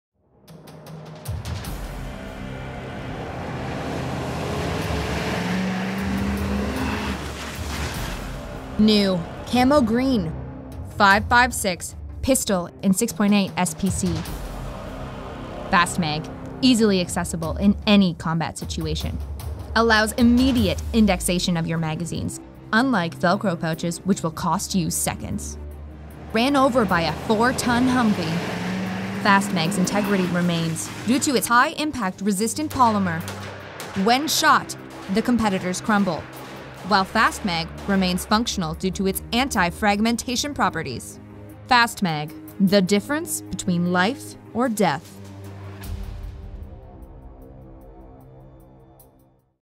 Fast Mag Gen IV - NEW Camo Green
Showcasing Hudson Tactical’s newest addition of the Fast Mag Gen IV in Camo Green. The product was brought into the shooting range to show its anti-fragmentation properties. A 5-ton HUMVEE runs it over while the Fast Mag remains operable.